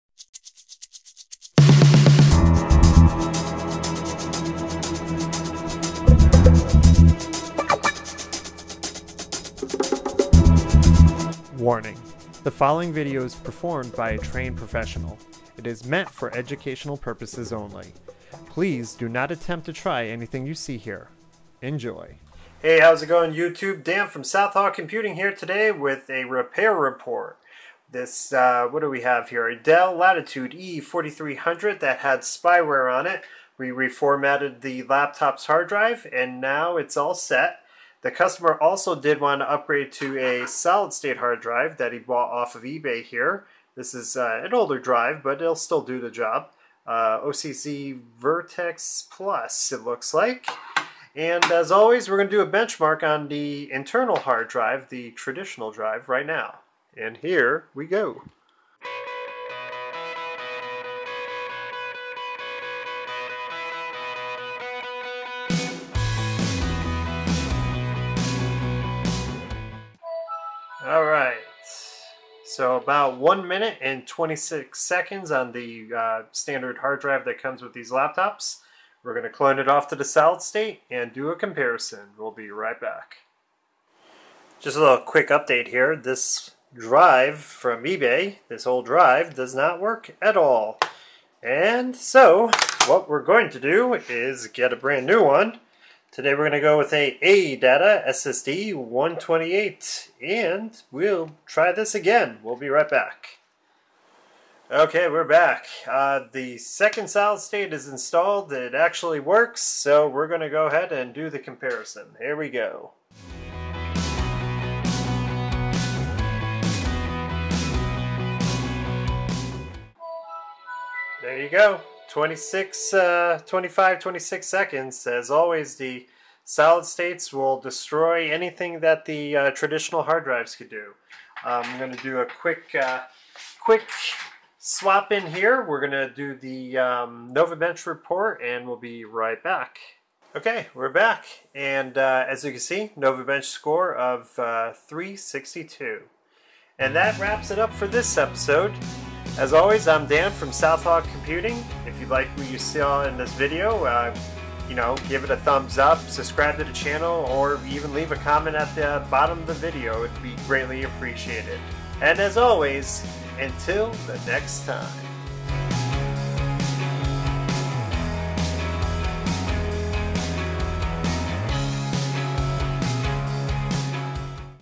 Tune Up Report: Dell Latitude E4300 Spyware/Solid State Drive Upgrade
This is a repair for a Dell Latitude E4300. This laptop had spyware, was reformatted and installed ADATA Premier SP600 ASP600S3-128GM-C 2.5" 128GB SATA III MLC. The laptop's NovaBench score was 362.

What is South Hawk Computing and what is the channel about? South Hawk Computing is a computer repair business. This channel for displaying repair/tuneup reports, tech news, and tech tips.